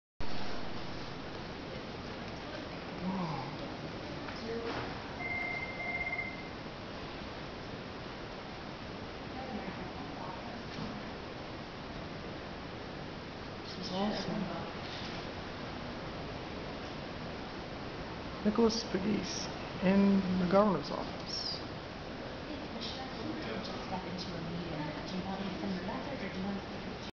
Inside the Office of the Governor 2012
CNN iReporter Nicholas Pegues is sharing with the YouTube Community inside Governor Bill Haslam's Office at the Tennessee State Capitol.